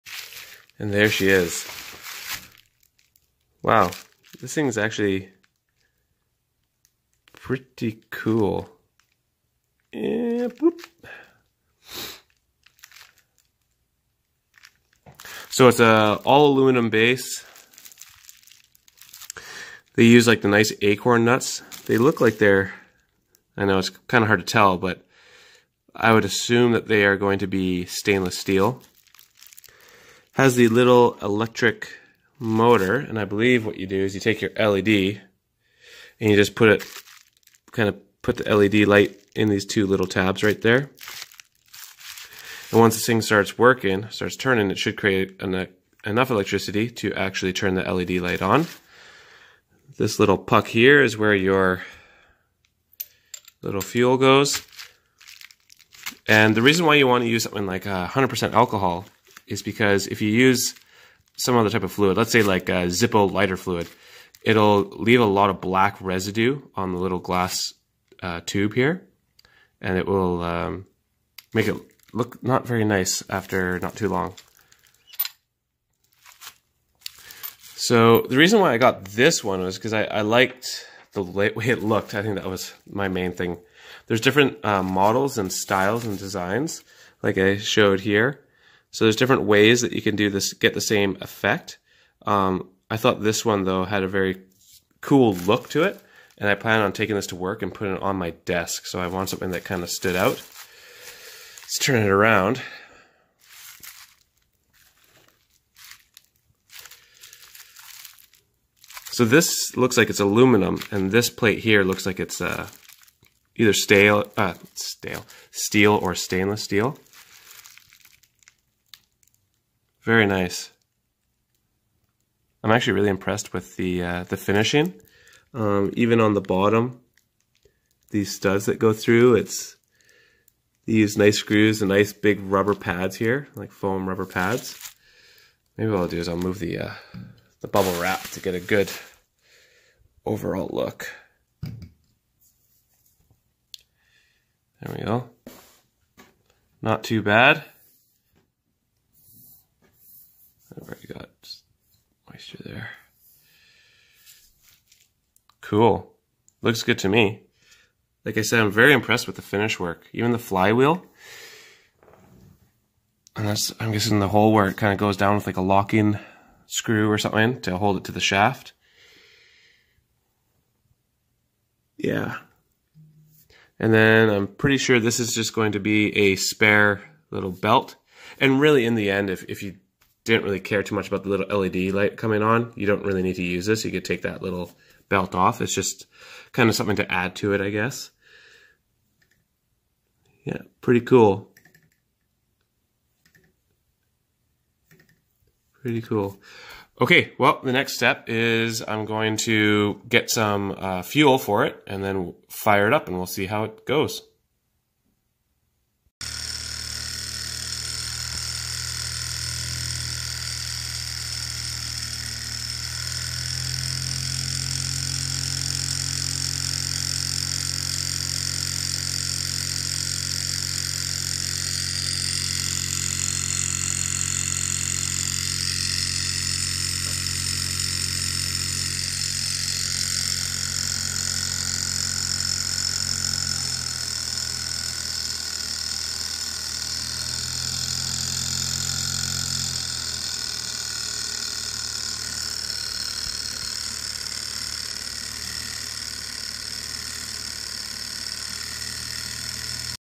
2019 nouveau générateur de cylindre de cuivre Stirling
Voici un gadget exceptionnel et incontournable pour tous les passionnés de science. C'est l'une des pièces magnifiques que la science a inventées jusqu'à présent. Conception innovant, processus de production rigoureux. Le principe Stirling est utilisé pour produire un petit modèle d'éducation et de divertissement.